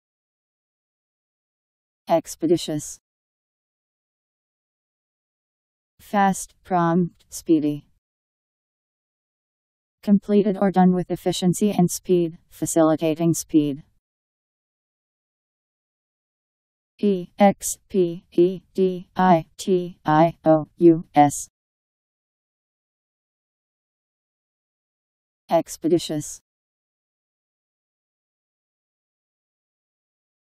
Expeditious Meaning
Video shows what expeditious means. Fast, prompt, speedy.. Completed or done with efficiency and speed; facilitating speed..  Expeditious Meaning. How to pronounce, definition audio dictionary. How to say expeditious. Powered by MaryTTS, Wiktionary